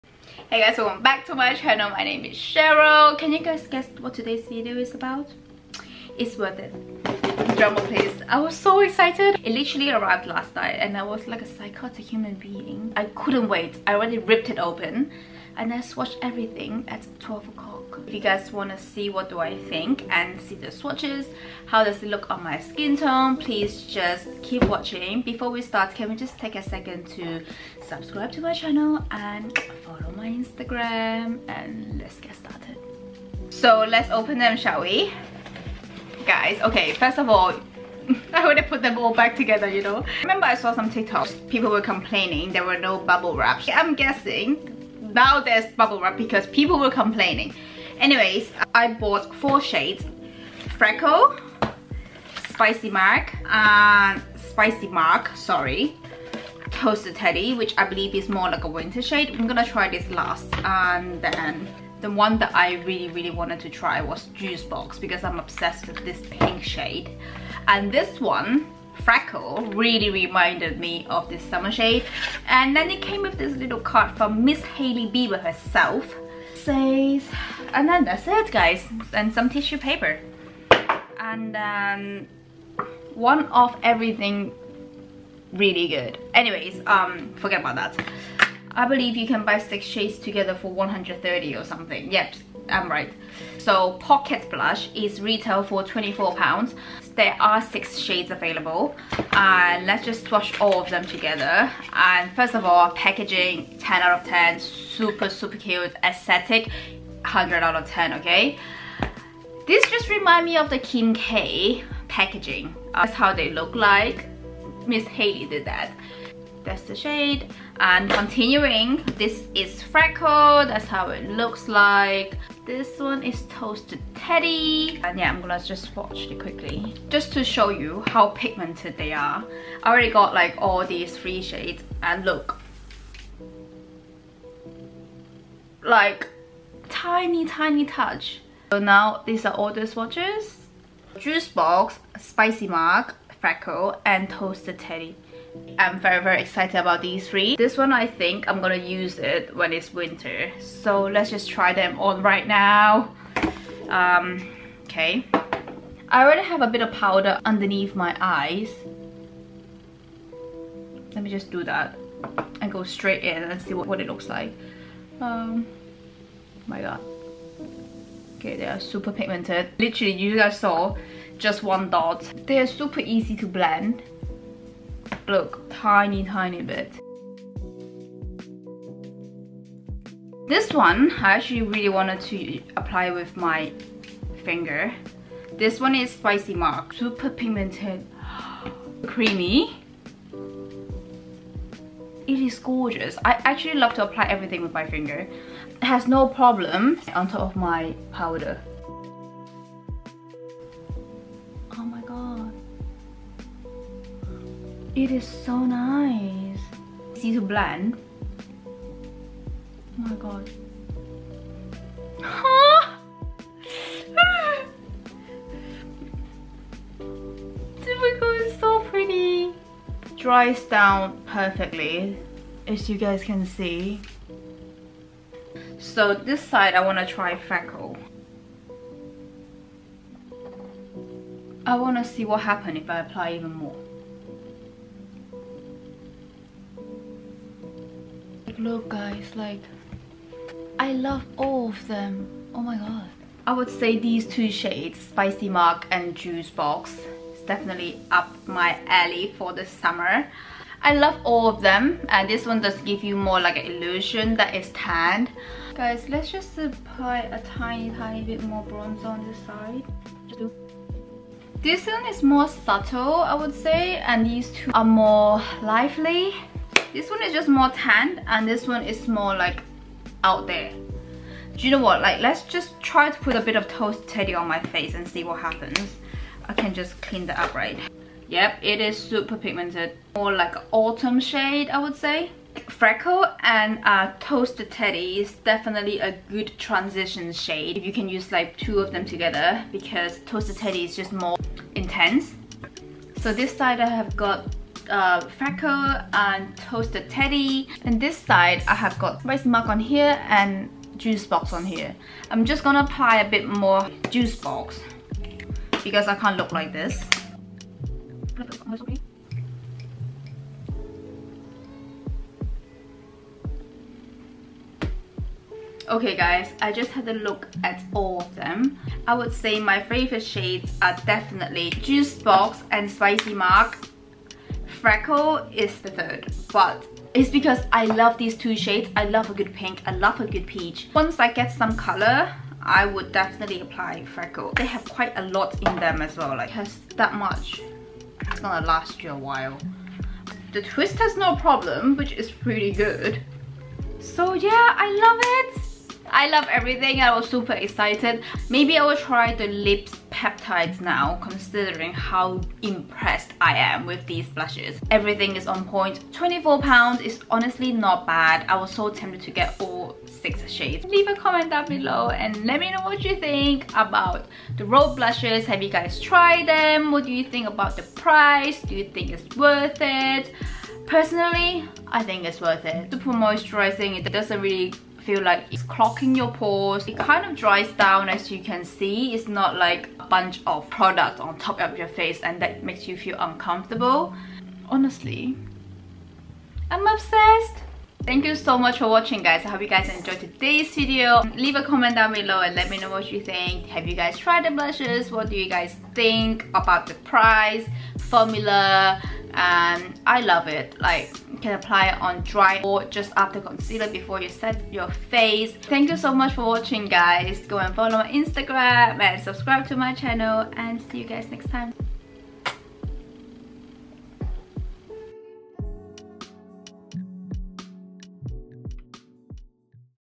NEW💕RHODE POCKET BLUSH FIRST IMPRESSION & REVIEW | ASIAN SKINTONE
Hi guys, in this video Im sharing my thoughts on the Rhode blushes.
It's the first time I purchased Rhode products!

This will be a first impression, swatches and review video.
I will also be sharing my favourite shade, if you have a similar skin tone to me it could be a good reference.

In today's video, We are diving into a first impression and review of the NEW Rhode Pocket Blushes, perfect for those with Asian skin tones.
I’ll be swatching the popular shades like Juice Box, Spicy Marg, Freckle, and Toasted Teddy to see how they perform and complement our unique undertones.

Join me as I explore the minimalist makeup approach with these pocket cosmetics, which are designed for on-the-go touch-ups. I’ll discuss the texture, pigmentation, and blendability of each shade, giving you all the insights you need before making a purchase. Is the Rhode blush worth the hype? Let’s find out together!

Whether you’re curious about Rhode blushes prices or tips on using pocket blush effectively, this review has got you covered.